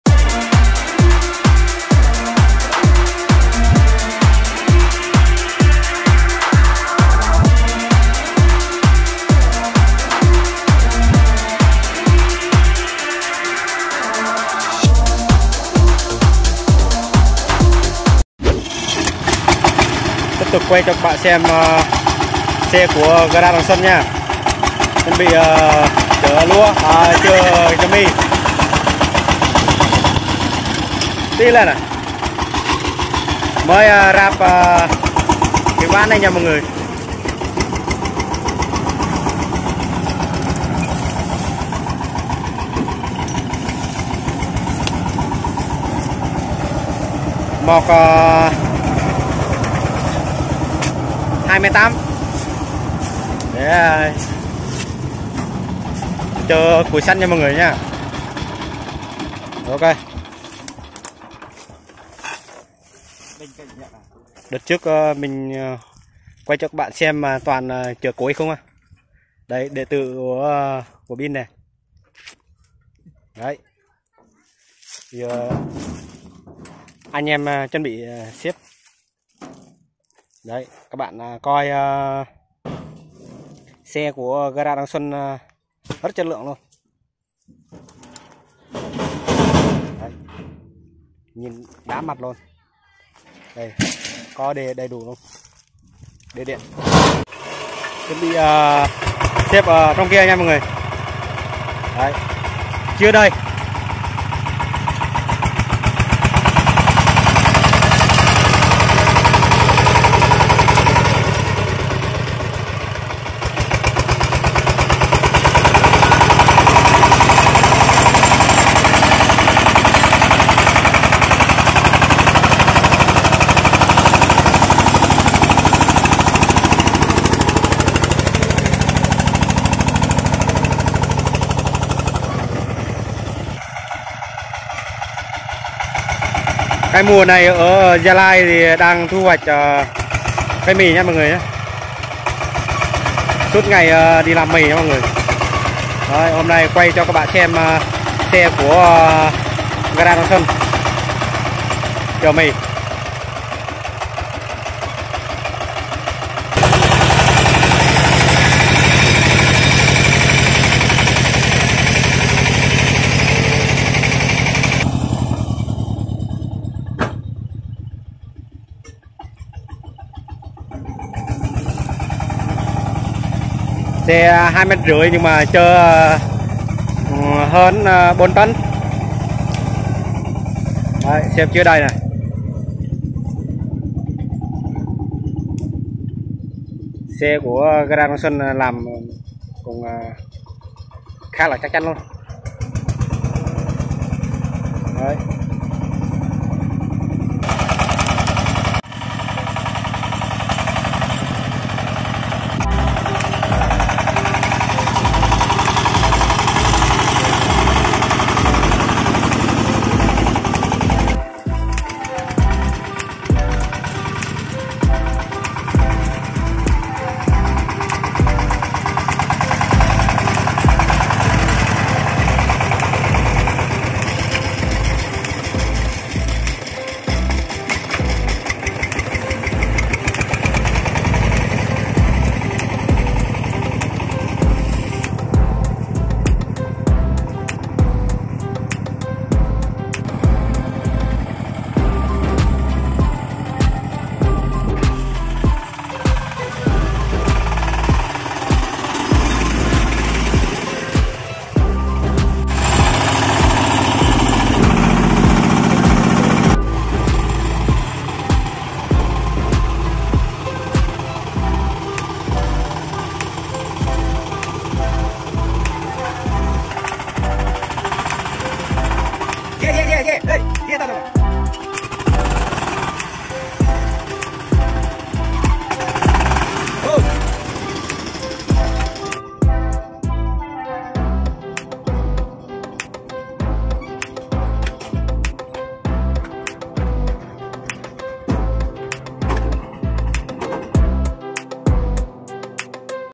Xe công nông đồng xuân chở mì xem mà khỏi chê luôn /nông dân thu hoạch mì 2021
Mọi người xem xe công nông chở mì nè đến mùa nhổ mì của bà con nông dân tại gia Lai lúc nào cũng gắn với xe công nông